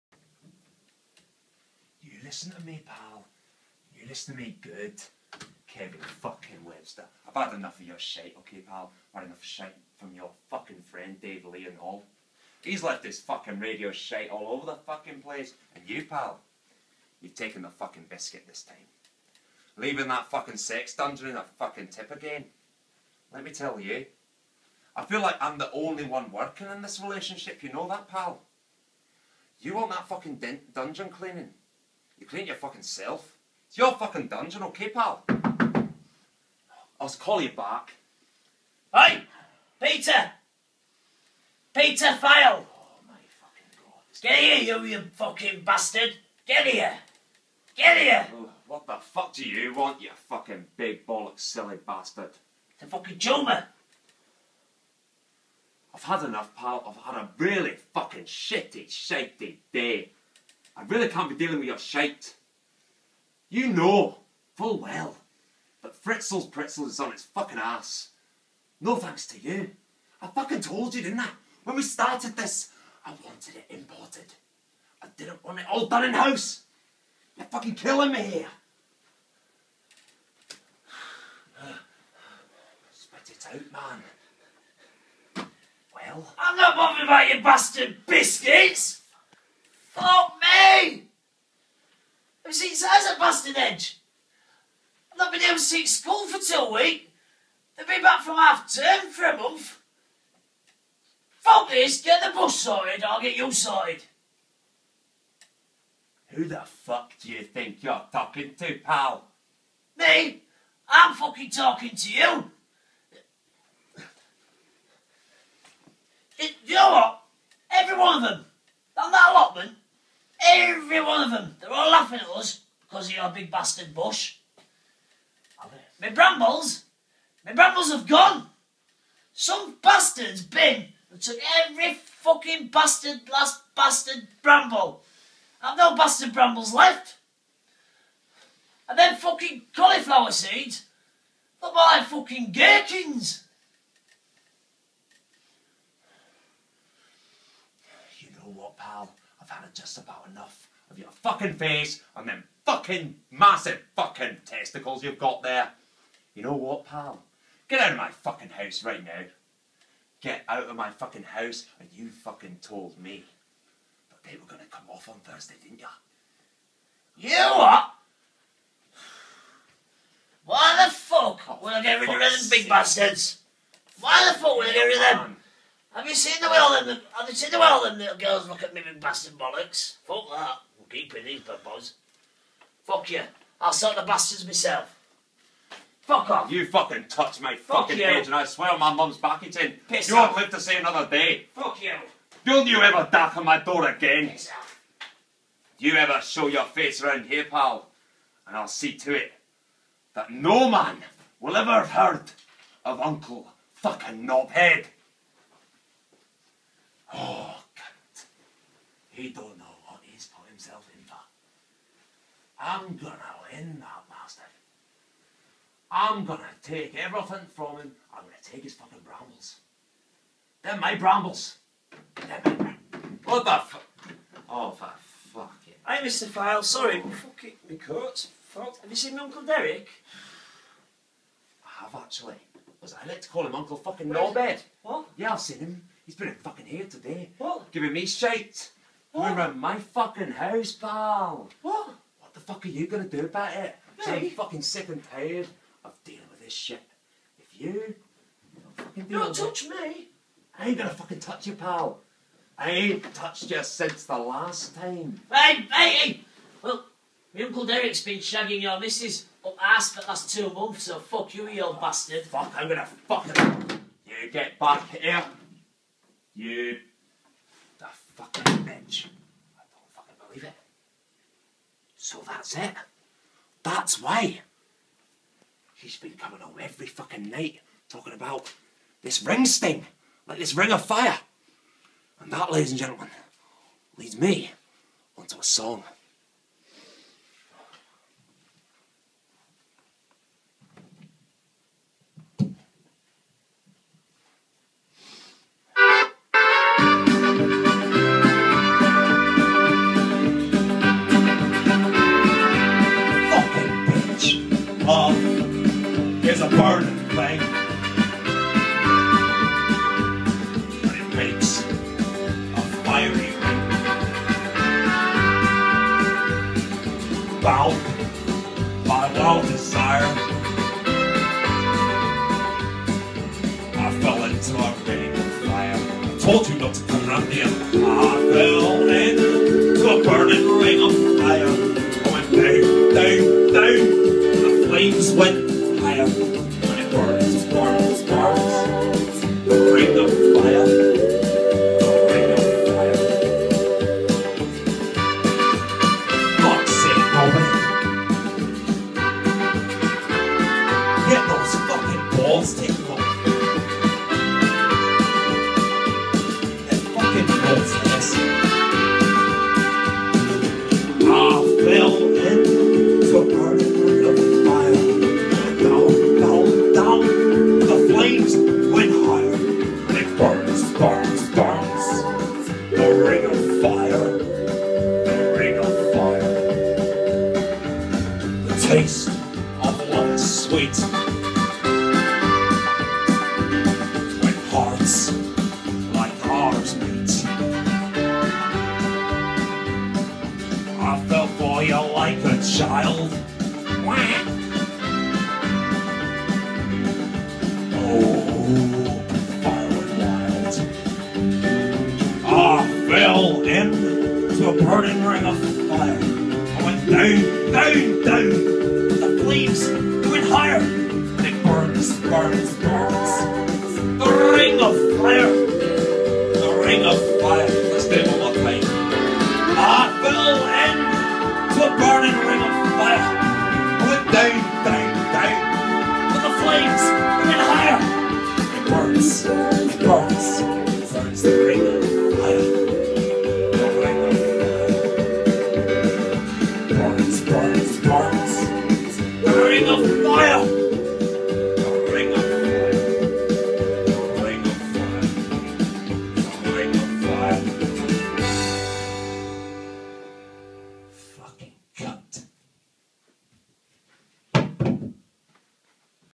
Uncle Knobhead Vs. Peter Phile Ep. 01 - The Ring Of Fire
NOT FOR THE EASILY OFFENDED

After cleaning out his friends Sex Dungeon, Peter gets into an argument with Uncle Knobhead about the height of his side of the garden hedge and the state of the company they founded together 'Fritzl's Pretzls' ... This leads to a horrifying discovery for Peter .